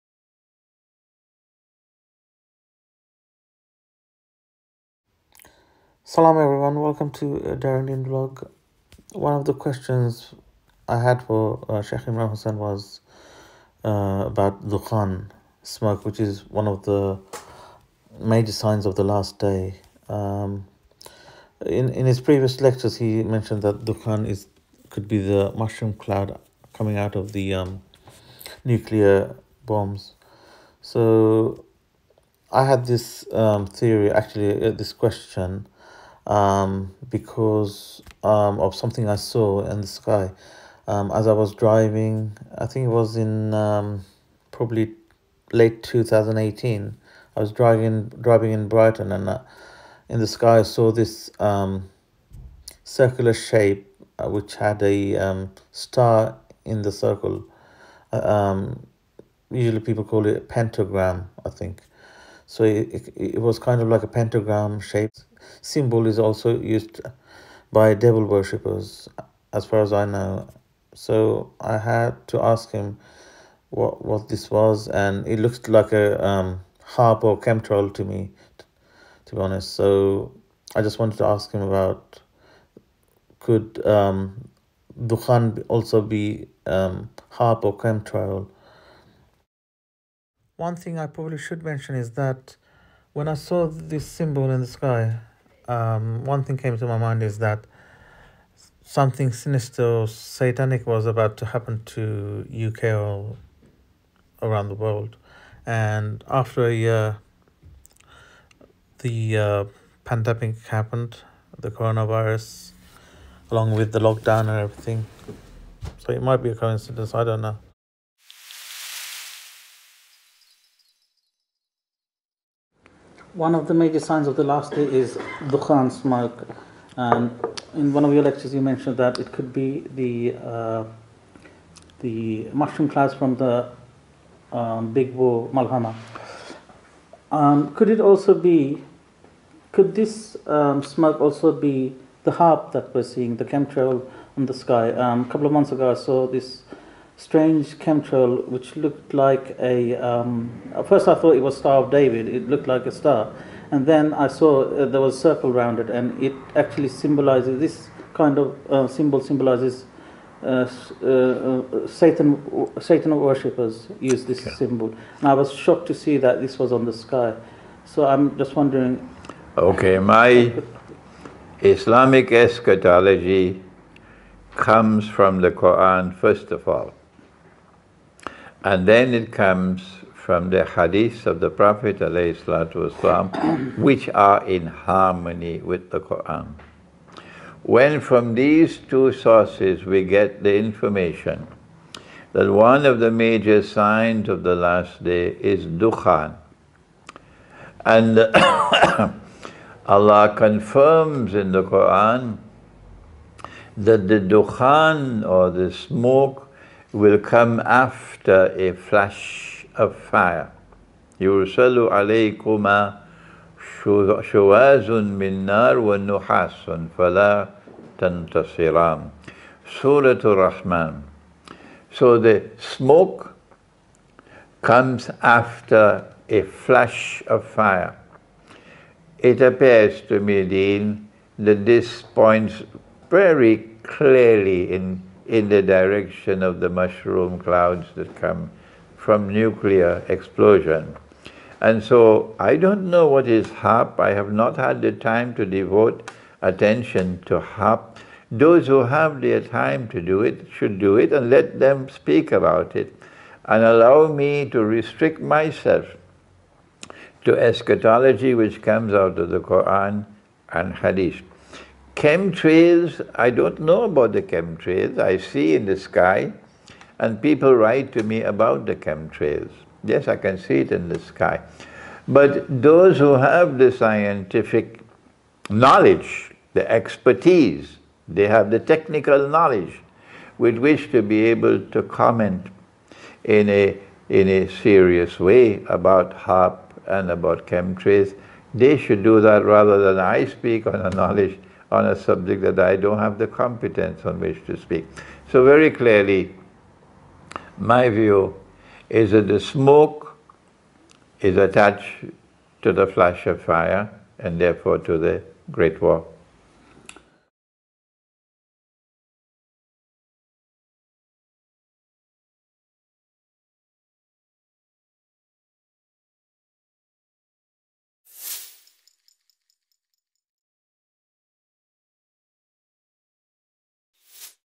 Major Signs of End Times Dukhan Smoke & What I Saw in the Sky : Interview with Sheikh Imran Hosein
Back in late 2018… as I was driving in Brighton, UK I noticed a white cloud in the shape of a Symbol of the devil worshipers, it looked to me like the star of david, and a circle around it (Pentagram). This brings me to my next question; One of the Major signs of the last day is Dukhan/Smoke. You once said the smoke could be the Mushroom Clouds from the Malhama or something else?
Interview with Sheikh Imran Hosein By Deen Choudhury on DaringDeen Vlog.

Recorded & Edited By
DaringDeen Vlog